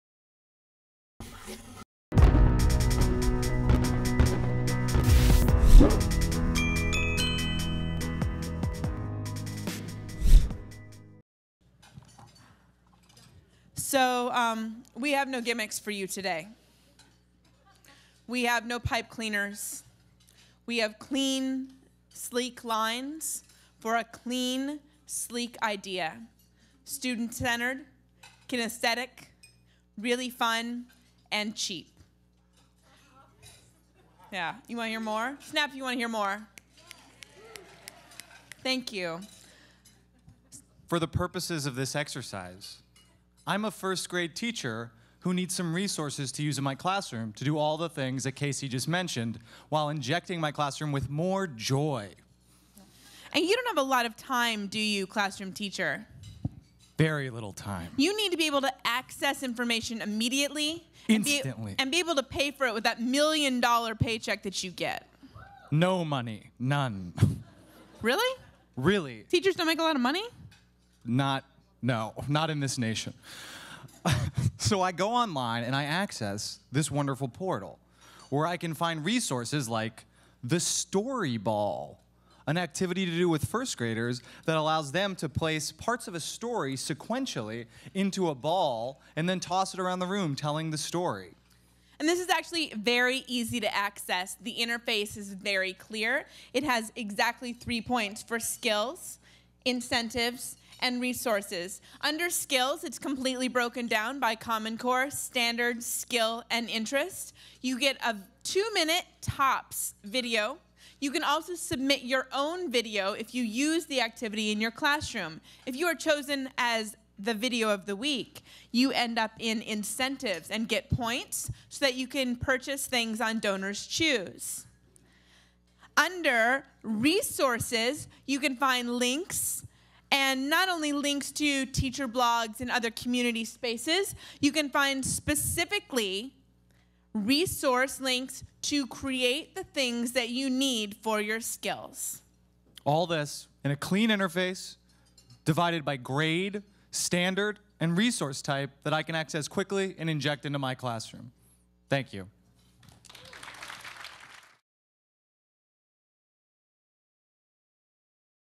Action Collab Prototype Demo: DIY Toolkit | BIF 2011
Design Challenge: How might we achieve universal competency in basic literacy and math skills?
The DIY Toolkit prototype is a library of videos demonstrating kinesthetic exercises for classrooms. Videos are teacher developed, selected, and rated.